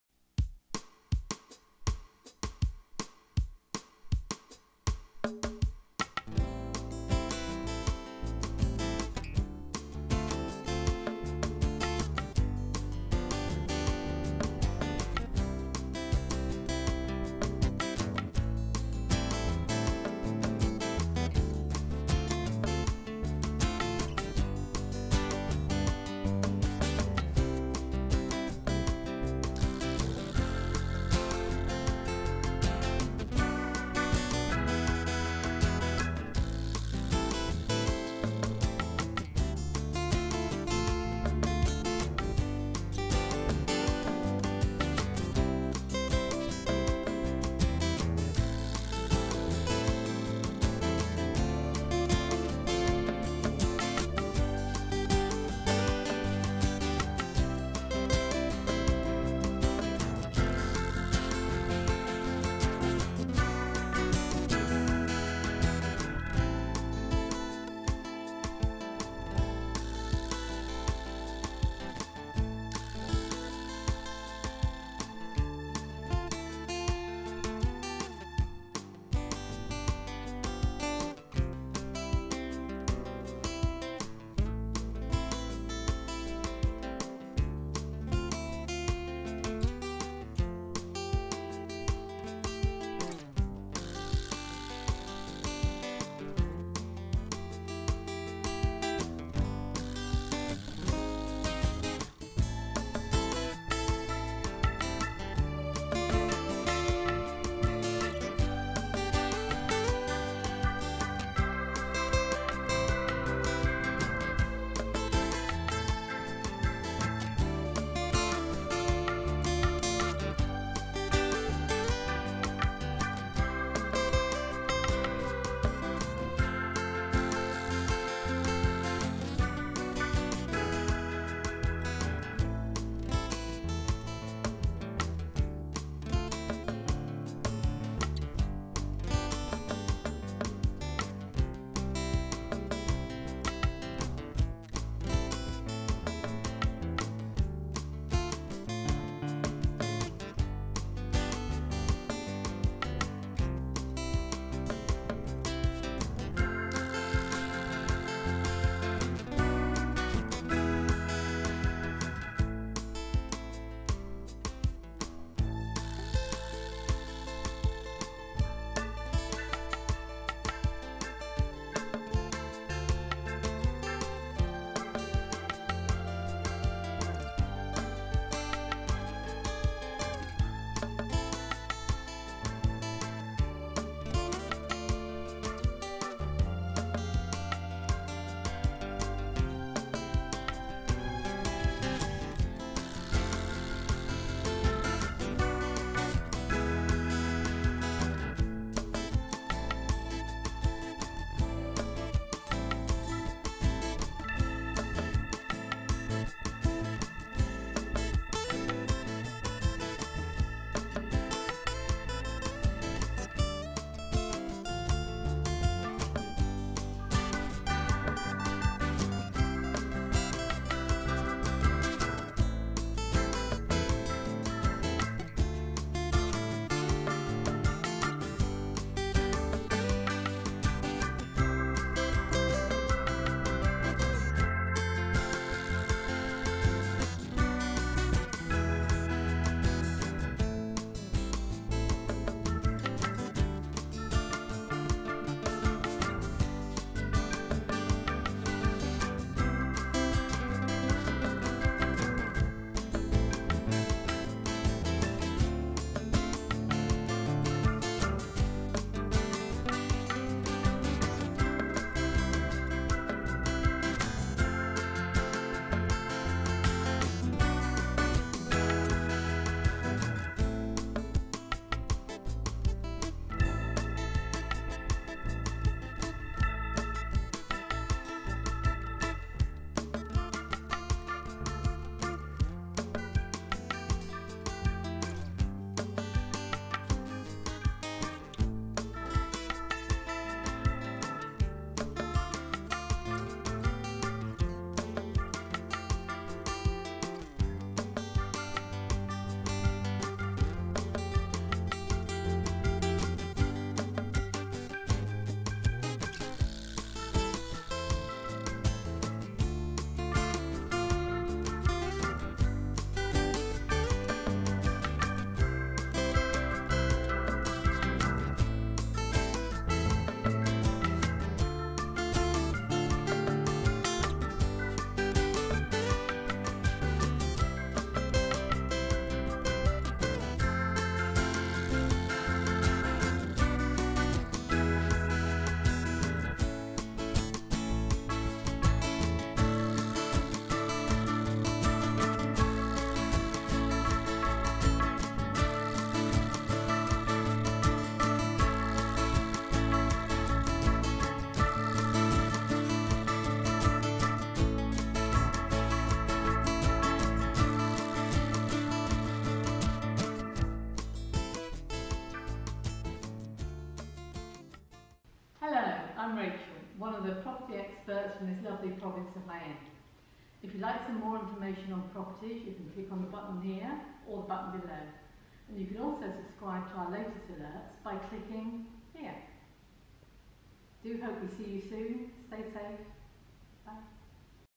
40K, Furnished 3 Bed 2 Bath + Terrace & Storage Property for sale in Spain inland Andalucia TH5696
Beautiful family home, with a generous 182m2 town plot, is situated in a traditional Spanish road in Martos in the Jaen province of Andalucia, Spain. As you enter the property you are greeted by generous bright entrance hall with a bathroom and stairs ahead and the lounge on the right. Up the stairs to your left and down the hallway is the first good size bedroom, to the right at the top of the stairs is the second bedroom with an ensuite bathroom and access to another room being an ample storage space with lots of potential . Back down to the ground floor, you enter the  lounge diner. Leading off the living room is an under-stair cupboard and access to a large 3rd bedroom and to the right is the kitchen. (Could easily be enlarged by removing the 3rd bedroom) this would make the kitchen three times bigger, with beautiful untainted views .The patio is situated directly off the kitchen. On the left are stairs that lead down to a smaller patio, with fruit trees and an open fireplace as well as some extra space that was used for animals. Under the house there is also a usable basement with electricity, made up of two rooms and a small storage area. Reduced, now on the market for 40,000 euros.